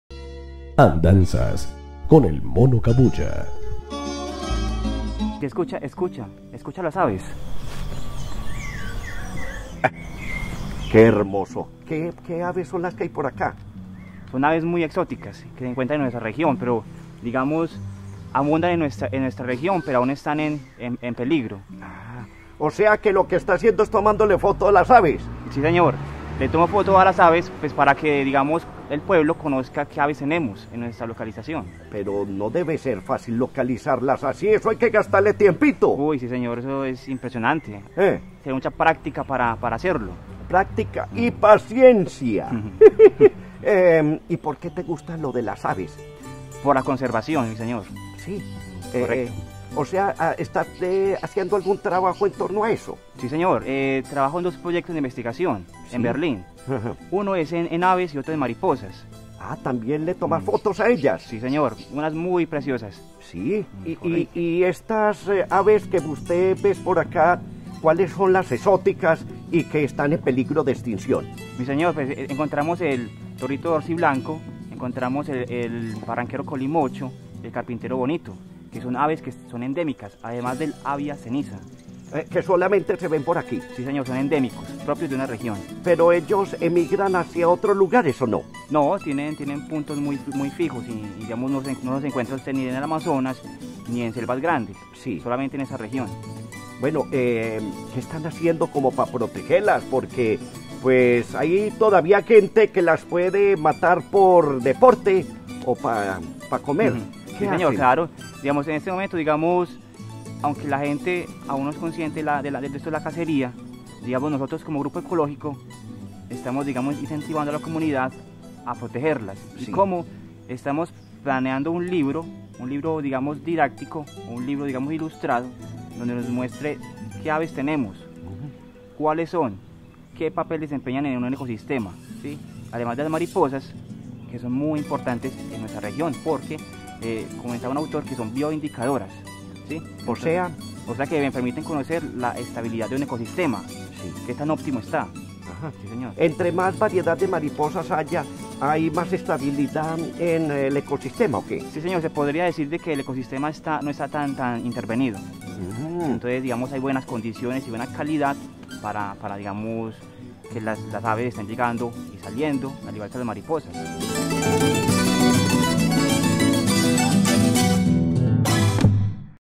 BERLIN SAMANÁ CALDAS AVES
Un especialista en aves nos explica qué clase de aves se encuentran en la región.